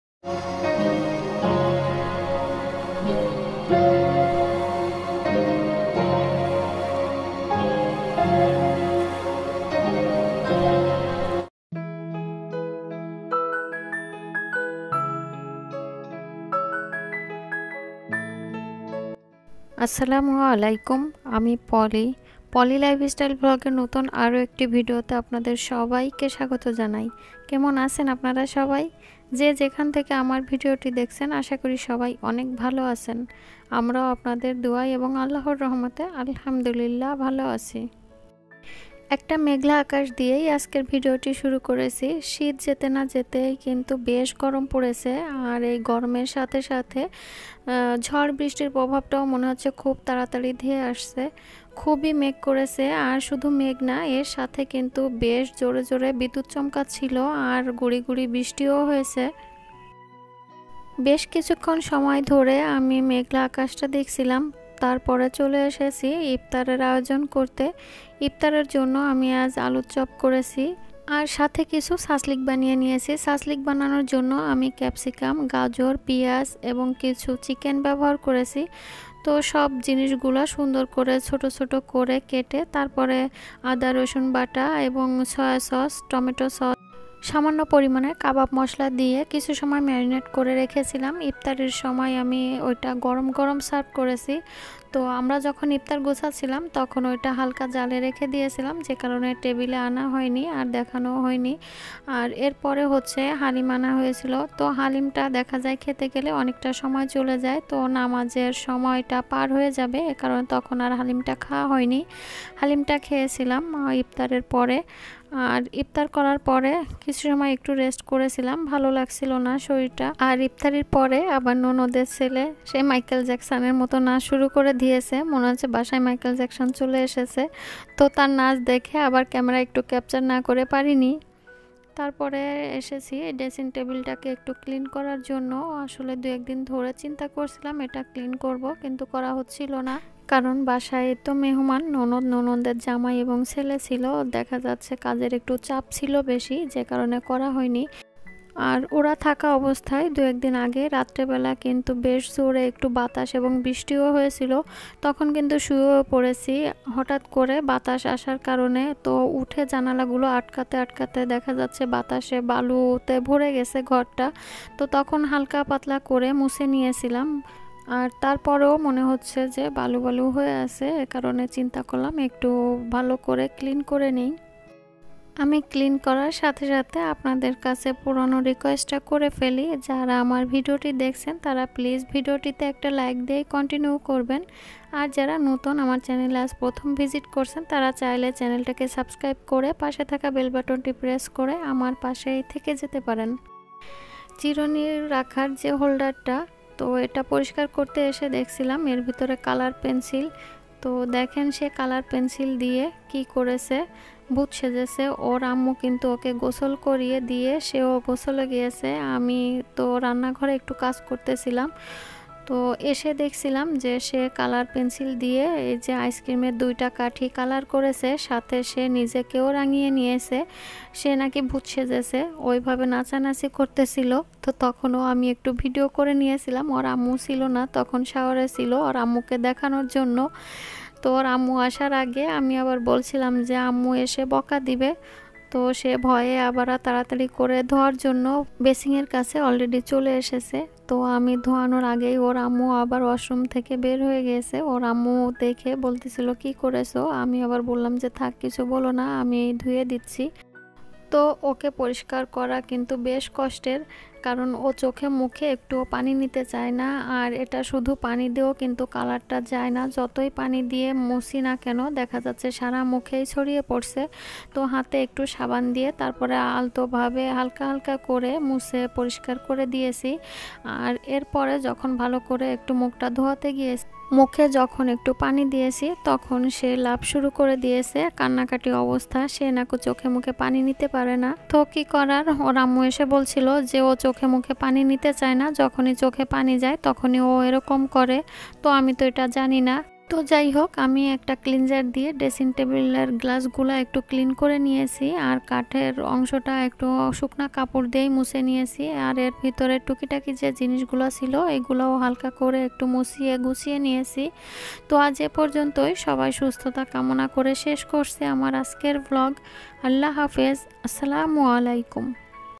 🍒ইফতার করে ড্রেসিংটেবিল ডিপক্লিন করে গুছিয়ে নিলাম।।@PoliLifestylevlog#dailyvlog
🍒ইফতারে আলুর চপ এবং সাসলিক বানিয়ে নিলাম।মাইকেল জ্যাক সানের নাচ দেখিয়ে দিলাম।

Today i have shared my daily life style vlog video.I have shared some cooking and cleaning activities at my house.I have shared my drasingtable cleaning and decoration.Hope you will love this vlog.

ifter preparation
dressing table decorated
drasing table cleaning
suslik recipe
chicken suslik

আসসালামু আলাইকুম,
                  নুতন আরও একটি ভিডিওতে আপনাদের সবাইকে শুভেচছা জানাই।আমার সারাদিনের টুকিটাকি কাজ নিয়ে আমার ভিডিও গুলো সাজিয়ে থাকি।আমার আজকের ভিডিওতে ইফতারের আয়োজন করে ড্রেসিংটেবিল ডিপক্লিন করে গুছিয়ে নিয়েছি।সেটাই শেয়ার করেছি।আশা করি আপনাদের ভালো লাগবে।আর ভালো লাগলে আমার ভিডিওটিতে একটা লাইক দিয়ে এবং চ্যানেলটাকে সাবস্ক্রাইব করে আমার পাশেই থেকে যাবেন ধন্যবাদ।